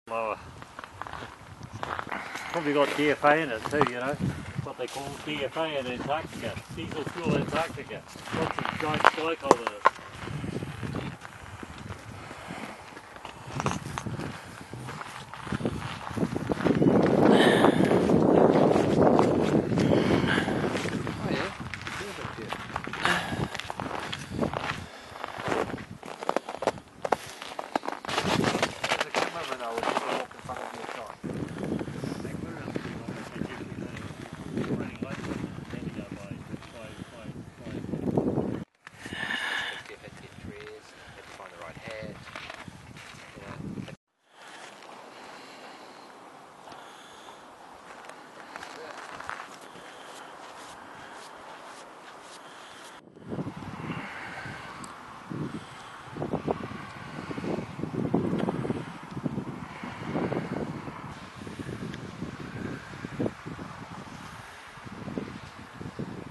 Truck falls through the Ice on frozen river in Siberia
A truck falls through the ice on the frozen Lena River in Yakutsk, in Siberia,  its minus 50 Celsius and the ice freezes again instantly.